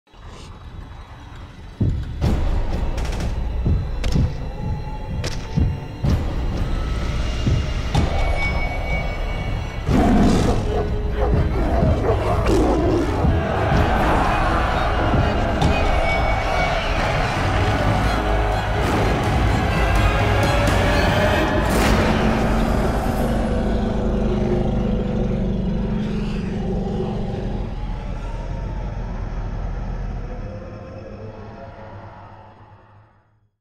Baltimore: The Red Kingdom Trailer
From writers Mike Mignola and Christopher Golden!

Lord Baltimore is missing. The Red King—evil incarnate—is taking over the world. Baltimore’s team has been driven apart, with some of them dying on the frontlines while others are in hiding. Since the day he set out for revenge on the vampire who killed his family, Baltimore’s fight has gotten bigger than he’d ever imagined, but what role will he take as evil finally threatens to consume the world?

On sale February 01, 2017!